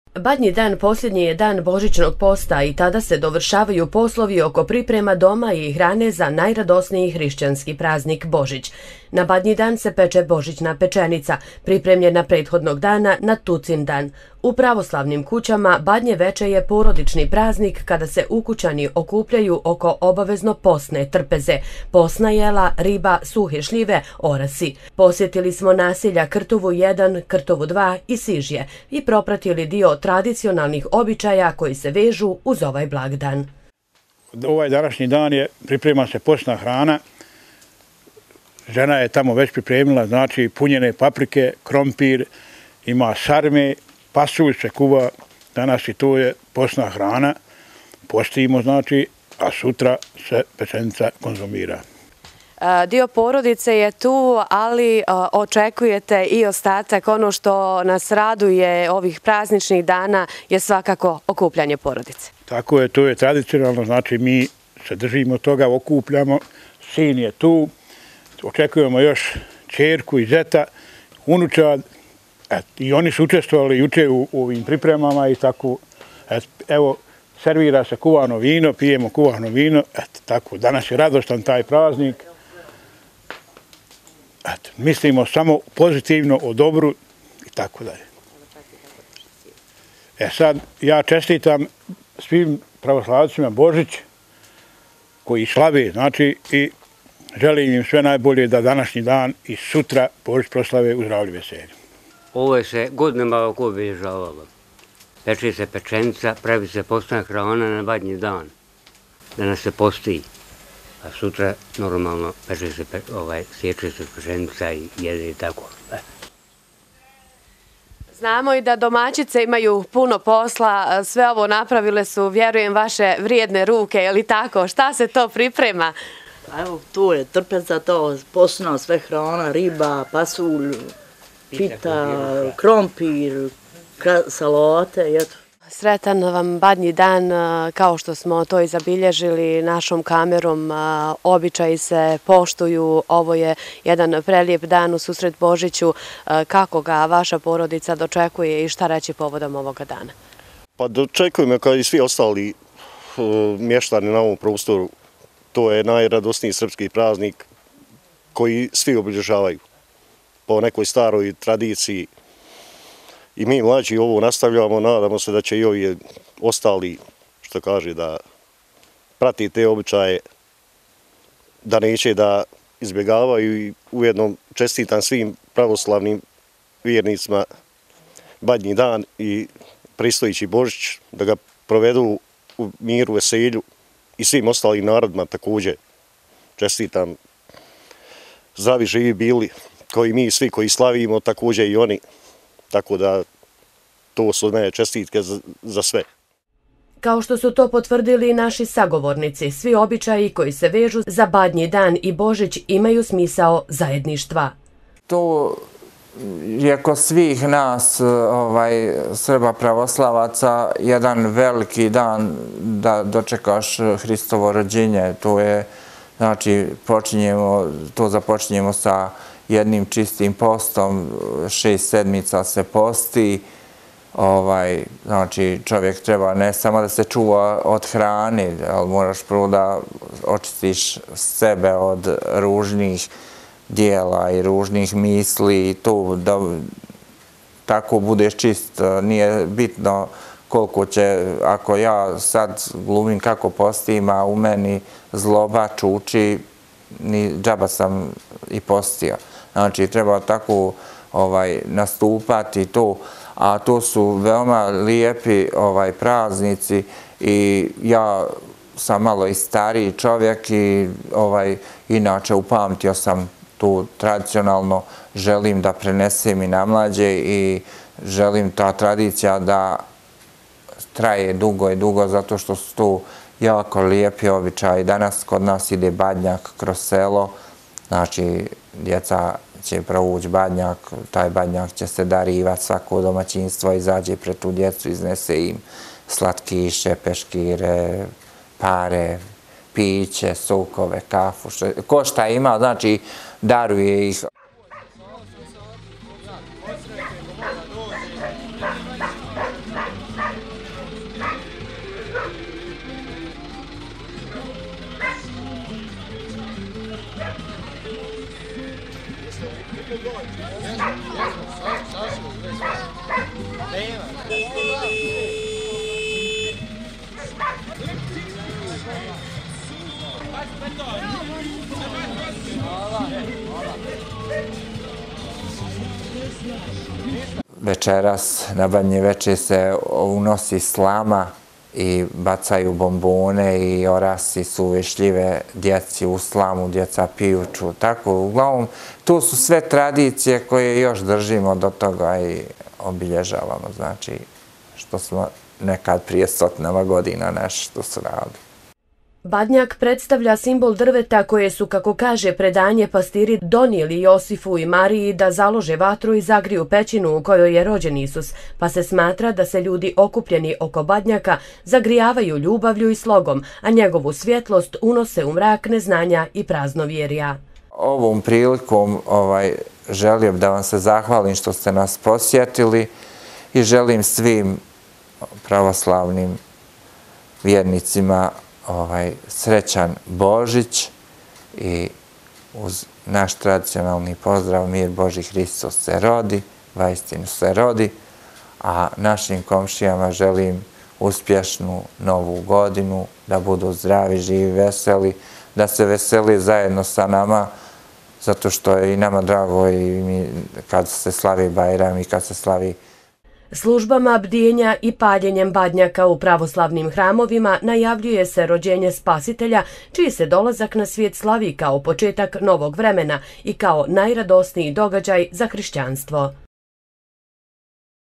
Badnji dan: RTV Lukavac u posjeti Krtovi i Sižju
Na Badnji dan se peče božićna pečenica, pripremljena prethodnog dana, na Tucindan. U pravoslavnim kućama Badnje veče je porodični praznik kada se ukućani okupljaju oko obavezno posne trpeze – posna jela, riba, suve šljive, orasi.  Posjetili smo naselja Krtovu 1 i Krtovu 2 i Sižje i propratili dio tradicionalnih običaja koji se vežu uz ovaj blagdan